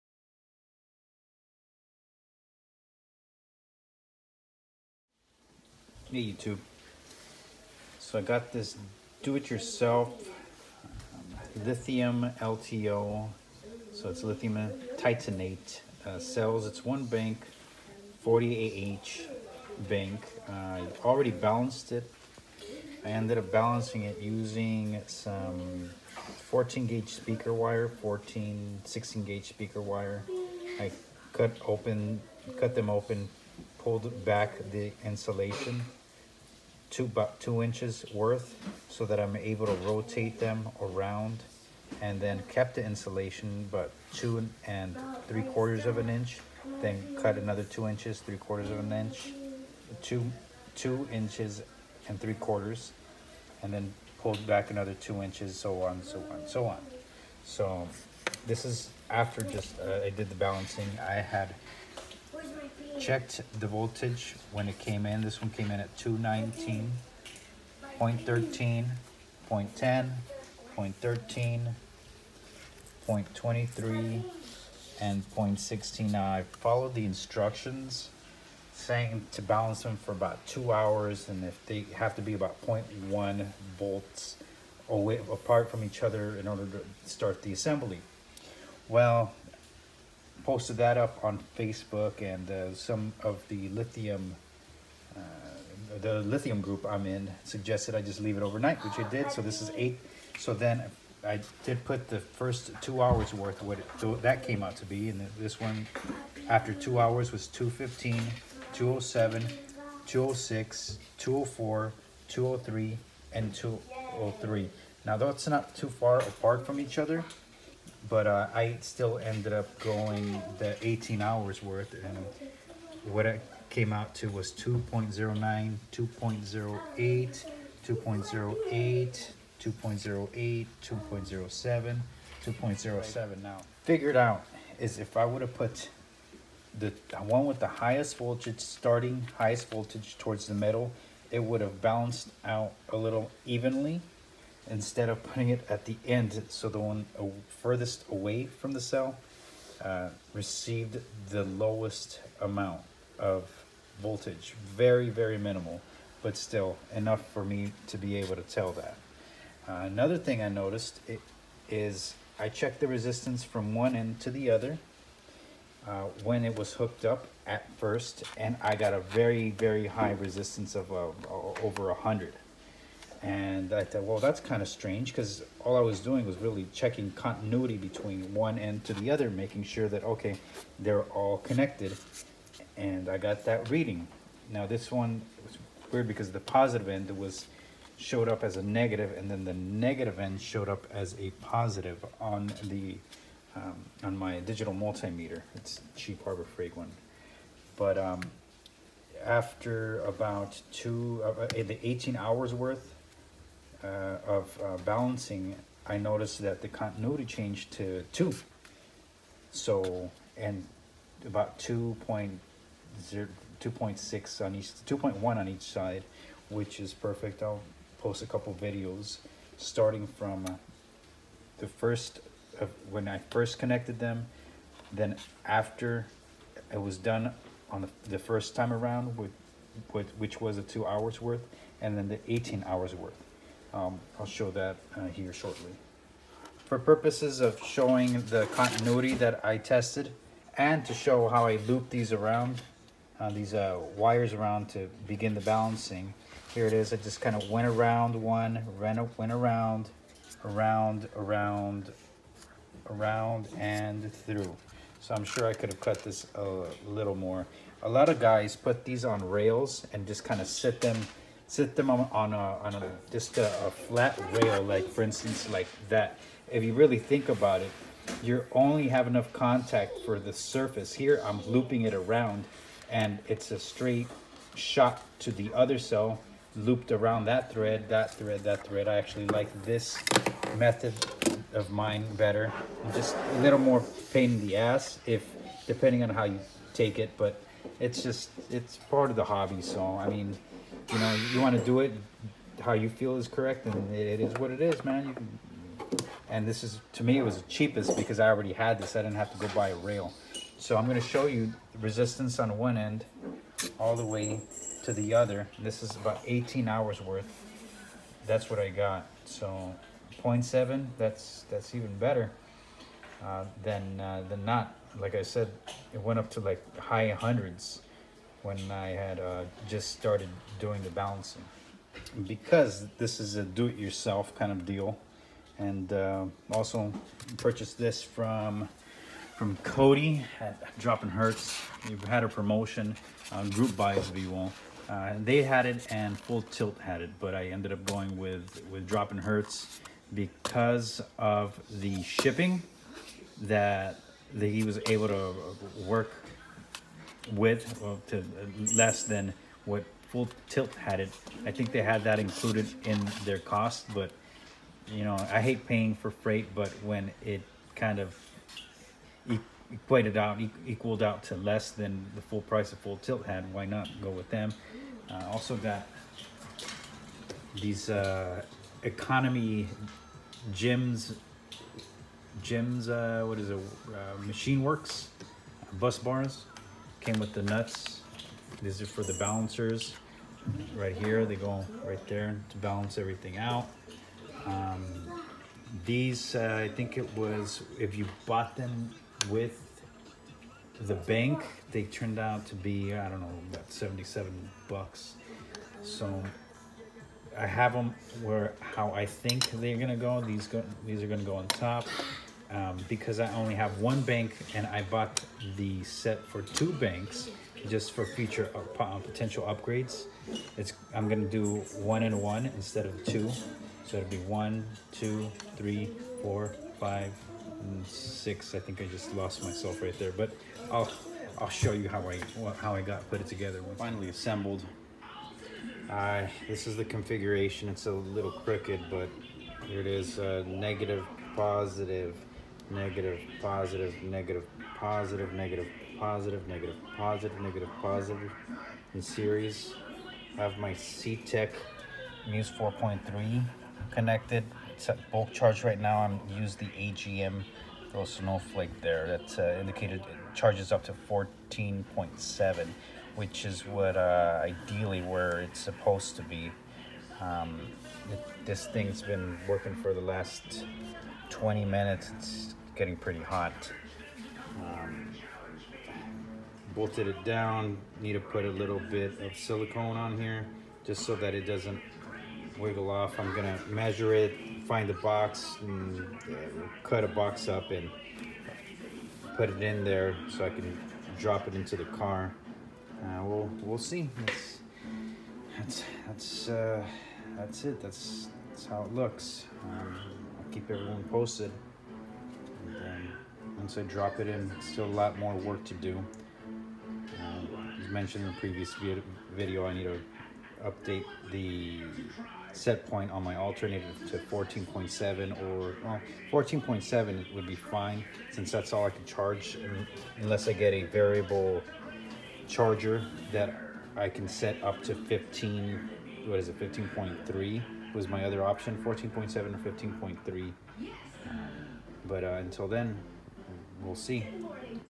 Lithium Titanate (LTO) tips to balance and assemble
Balancers still on their way.  Bussing was also purchased from DroppinHz at a discount with the purchase of their group buy.

Charger recommended:

DC Power Supply Variable,0-30 V / 0-10 A LW-K3010D Adjustable Switching Regulated Power Supply Digital,with Alligator Leads US Power Cord Used for Spectrophotometer and lab Equipment Repair